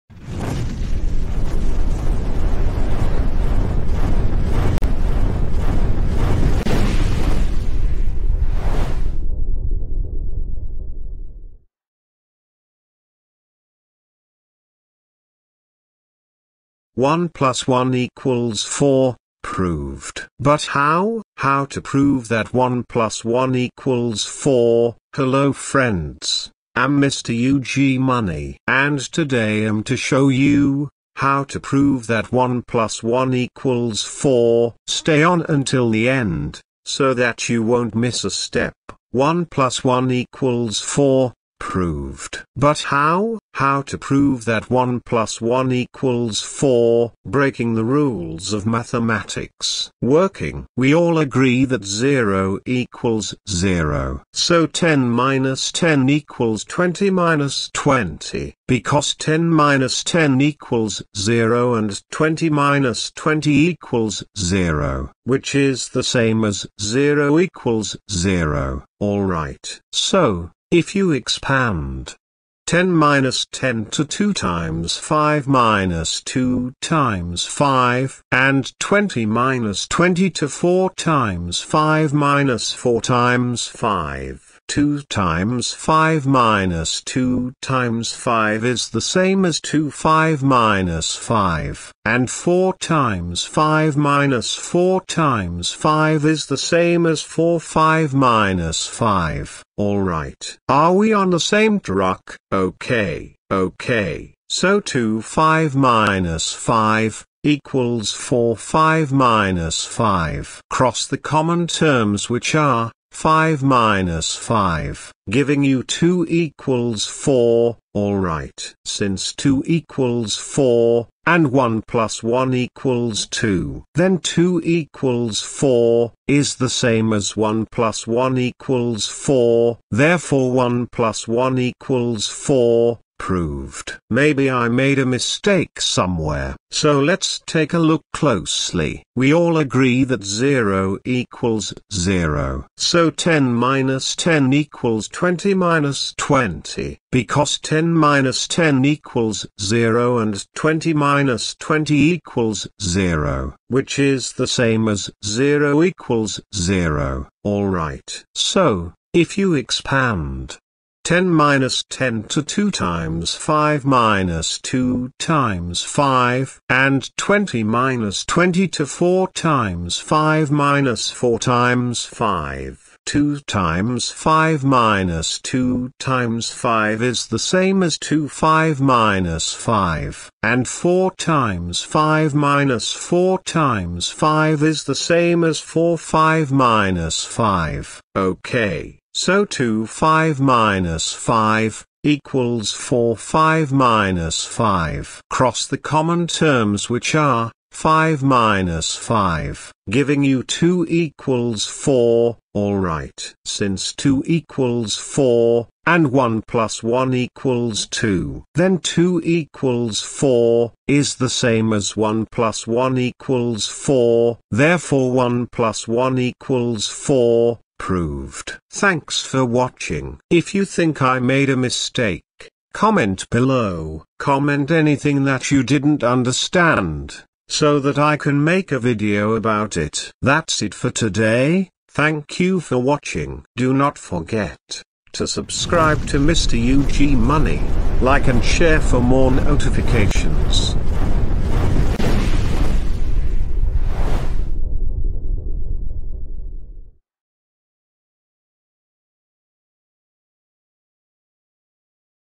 1 + 1 = 4 proved | How to prove that 1 + 1 = 4 | Breaking the rules of mathematics
1 + 1 = 4 proved | How to prove that 1 + 1 = 4 | Breaking the rules of mathematics

Hello, am Mr Ug Money.
Am Here To Help You.